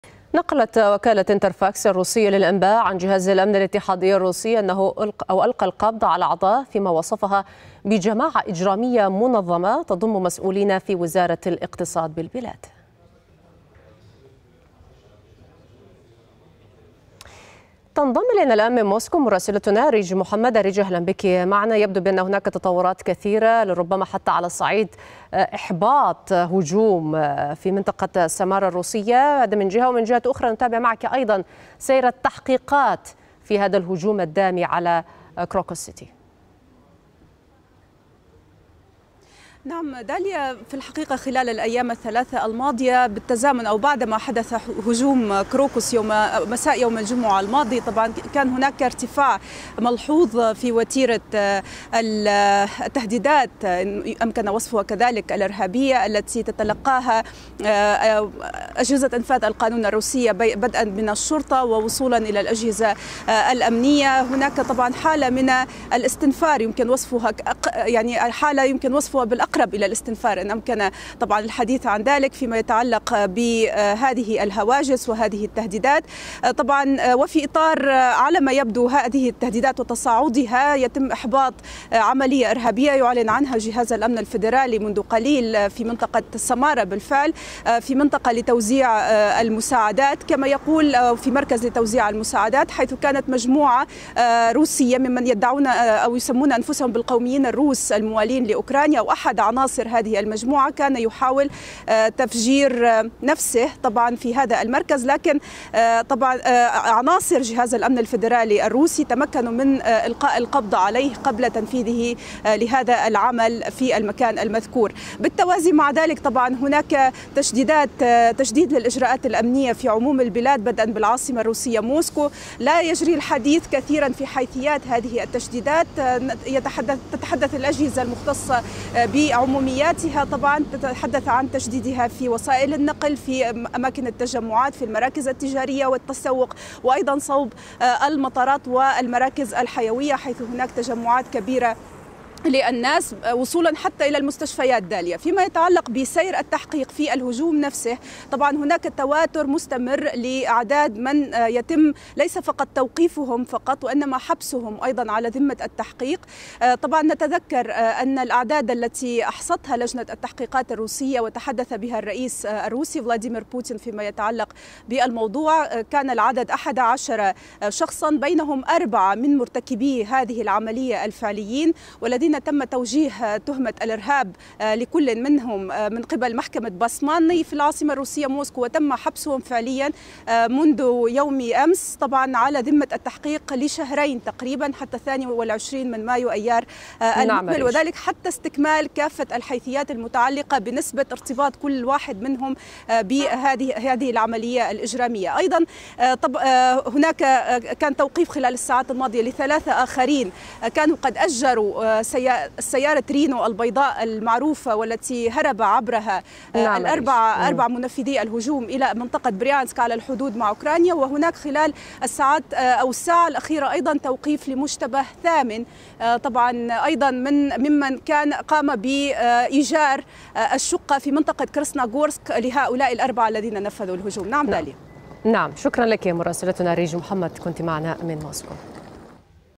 السلطات الروسية تقبض على جماعة إجرامية منظمة تضم مسؤولين بوزارة الاقتصاد
نقلت وكالة إنترفاكس الروسية للأنباء عن جهاز الأمن الاتحادي الروسي،
أنه ألقى القبض على أعضاء في ما وصفها بجماعة إجرامية منظمة تضم مسؤولين في وزارة الاقتصاد بالبلاد.